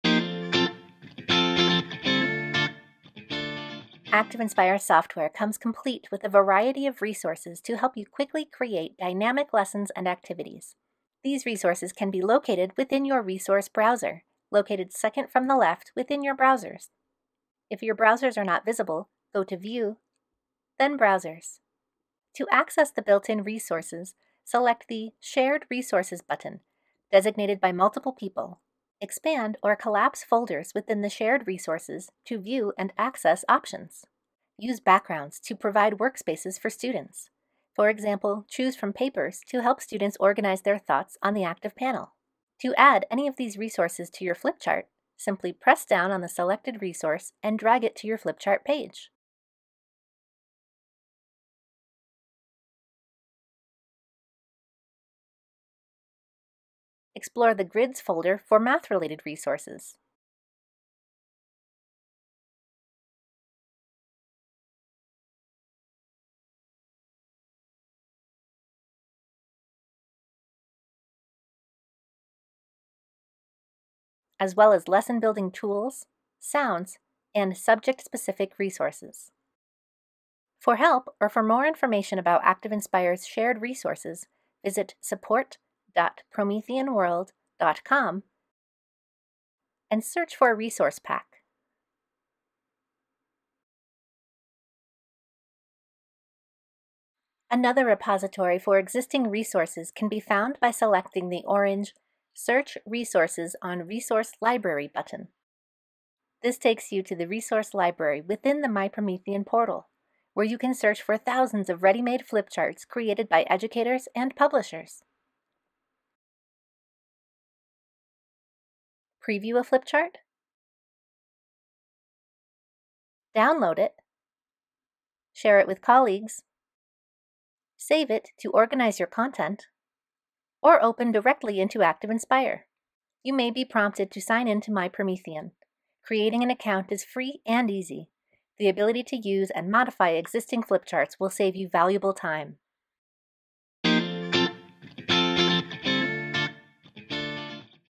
Learn Promethean: Resource Browser in ActivInspire
Learn about the Resource Browser in *ActivInspire.

*Software demonstrated may not represent the most updated version.